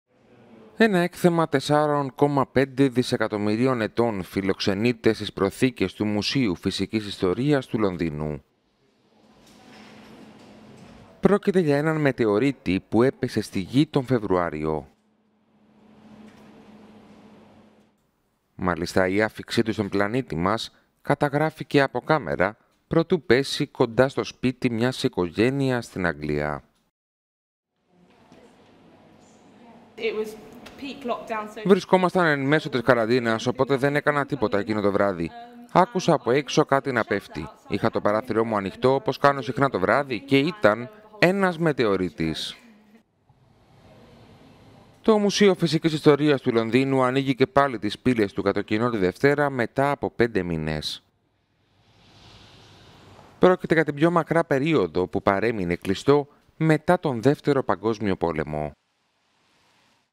Ένας μετεωρίτης 4,5 δισεκατομμυρίων ετών στο Λονδίνο
Βρίσκεται στο Μουσείο Φυσικής Ιστορίας της βρετανικής πρωτεύουσας